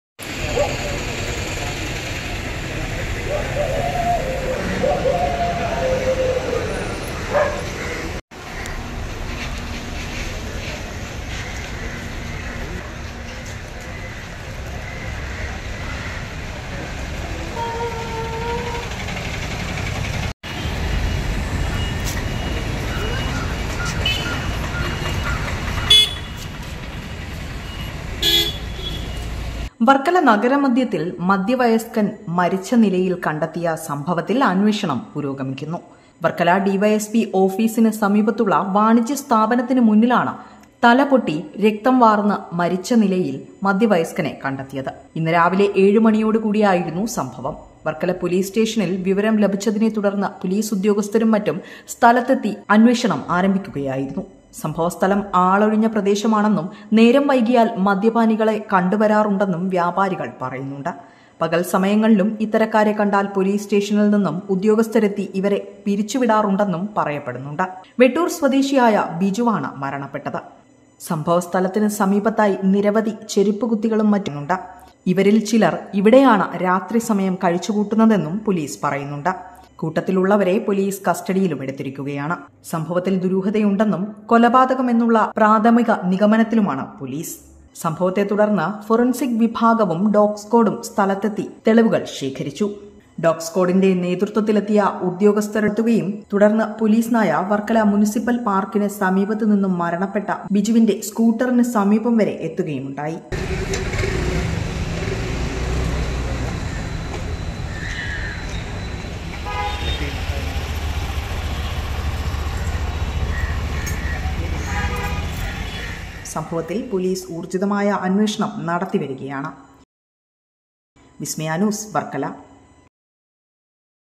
പോലീസുകാർ പോലും അറിഞ്ഞില്ല; കടതുറക്കാനെത്തിയ വ്യാപാരി കണ്ടത്!
പോലീസ് സ്റ്റേഷന് തൊട്ടടുത്ത്, എന്താകും നടന്നത്, ആരാകും ഇതിനു പിന്നിൽ